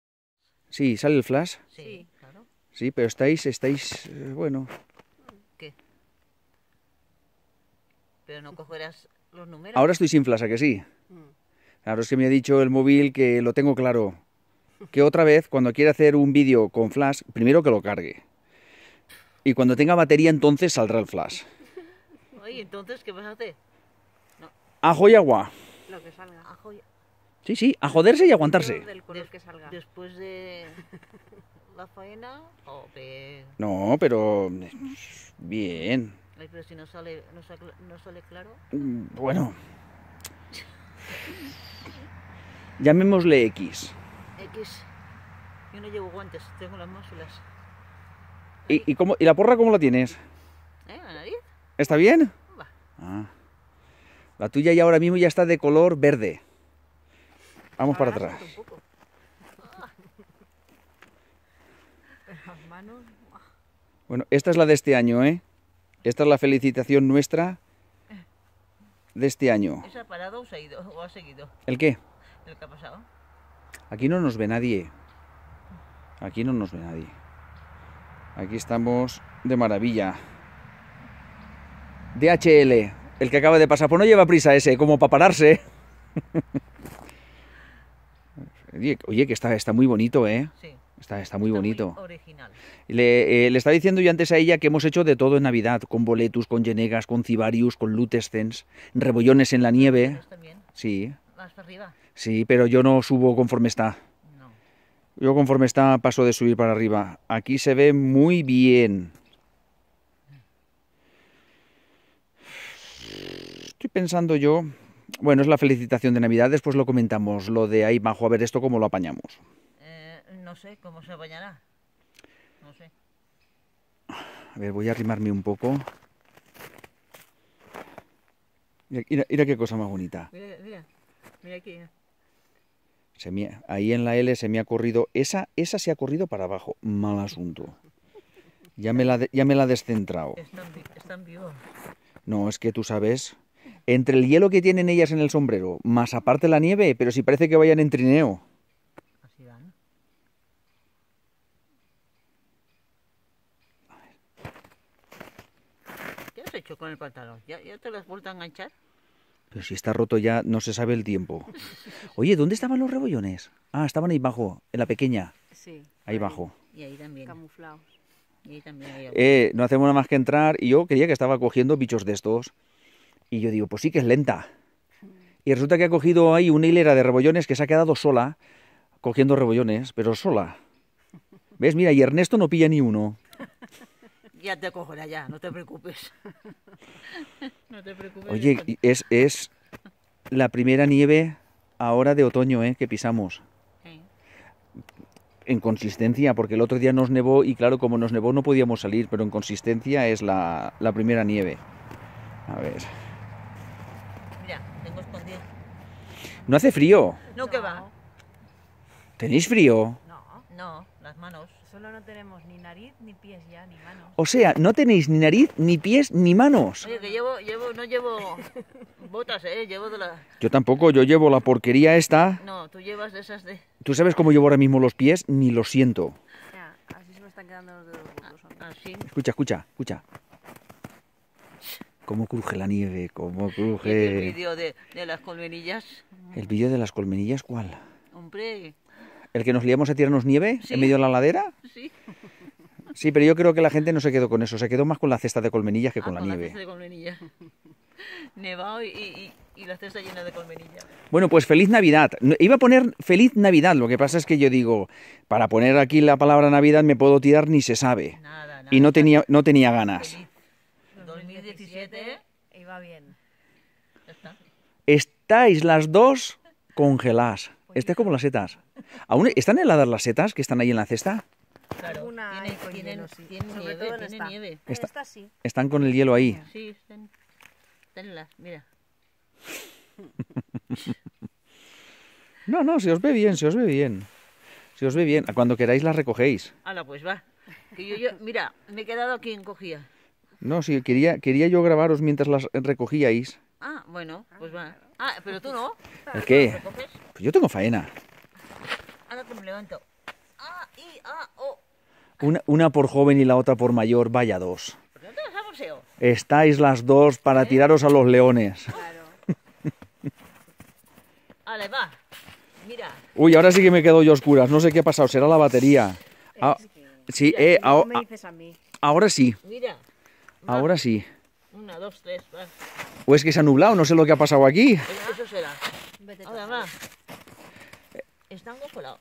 Feliz Navidad !!!!!!
Como no podía ser de otra manera la felicitación de este año y ya van muchos con las setas de por medio.
Aquí hay dos de las tres mujeres mas importantes de mi vida, falta mi hija que con toda seguridad le hubiera encantado haber estado con nosotros en la nieve y rodeada de setas, como siempre problemas burocráticos y de adultos lo han impedido.
Desde aquí y por este medio le felicito a la nena las Navidades ya que no me encuentro mas que con todo tipo de impedimentos a la hora de intentar hablar con ella por las tardes y cuando digo intentar ya sabéis por que lo digo se queda en eso en un mero intento, así que vida mía tu padre te desea una Feliz Navidad con todo mi cariño y amor por lo que representas tanto para mi como para tu abuela.